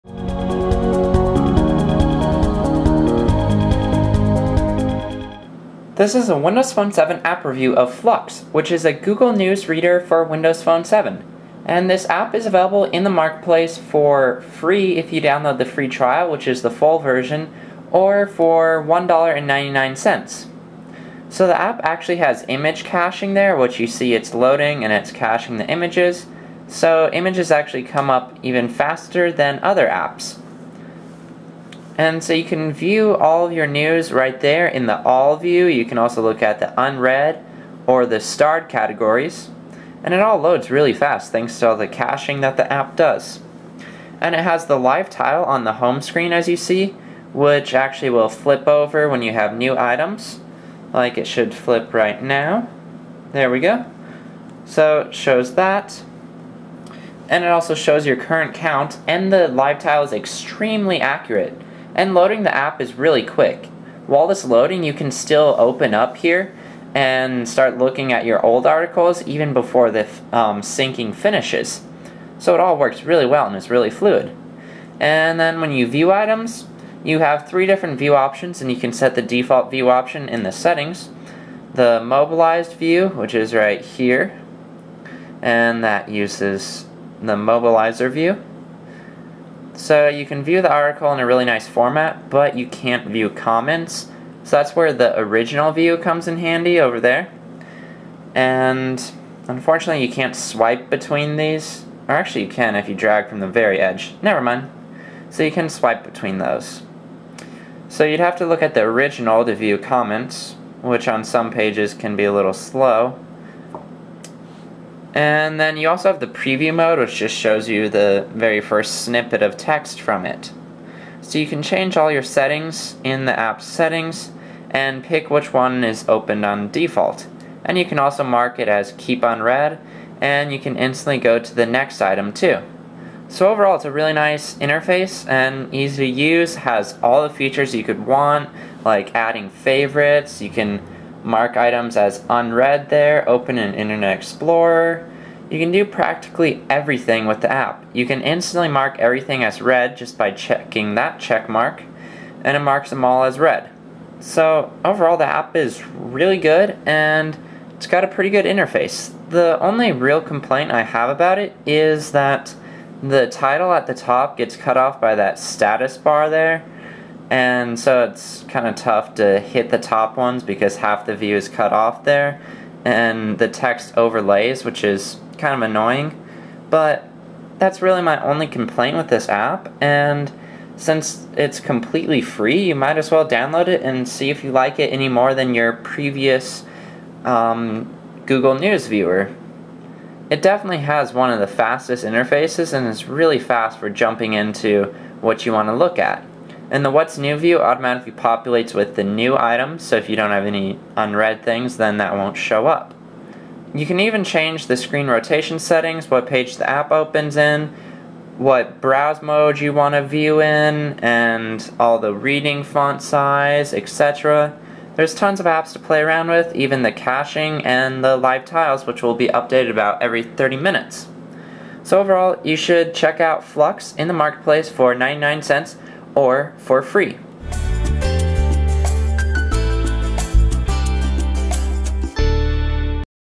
WP7 App Review: Flux (WMPowerUser.com)
This is a Windows Phone 7 app review of Flux, reviewed by Andrew Bares

There are already tons of Google Reader apps for WP7. Does this one stand a chance? Yes!

If you're looking for a free (or $1.99 too) Google Reader app that loads extremely fast, caches article data and images, and has a perfectly functioning live tile, Flux is a great choice.

One of the most important things about a news reader app is the ability to quickly open the app, go to your articles, and start reading. Flux makes this extremely fast, since you can open your news list while the app is still syncing changes! And all the images are cached, so things load extra fast.

Another important factor is reading the articles themselves. Flux has a preview, mobilized, and original view right in the same pivot menu, so it's easy to switch between all three.

The Live Tile is also an essential factor, and Flux's tile always works perfectly. It never has random glitches like some apps did, and always accurately represents how many new articles you have to read.

Finally, a reader app also has to have a great interface. Flux doesn't have the greatest main screen I've ever seen, but the content views with your articles (where you'll actually spend time in the app) are great. They show you the title, an icon image, and a quick snippet of the first part of the article. The interface is also very snappy, and allows you to do things like marking everything as read with one click. The status bar does unfortunately overlay on a lot of the text headers, but that's the only real issue I could find with the app.

Overall, Flux is one of the fastest and best Google Reader apps out there!

Title: Flux
Publisher: edonistart